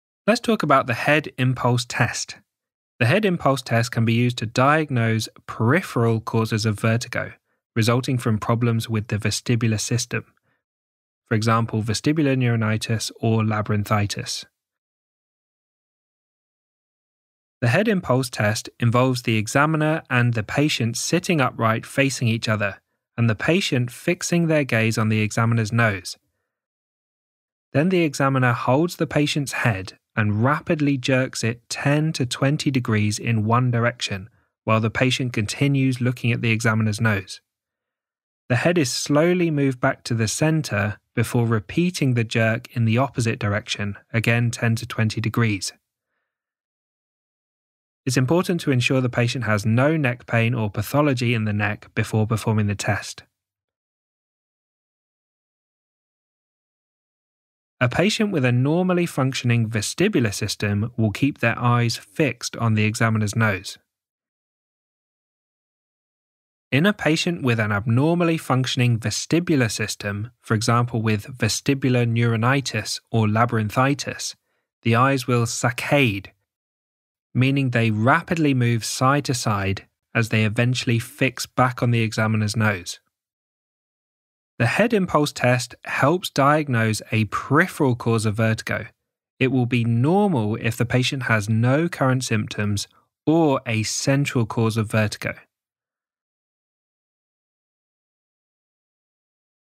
The Head Impulse Test
This video contains a visual explanation of the head impulse test, aimed at helping students of medicine and healthcare professionals prepare for exams.

Written notes on the head impulse test can be found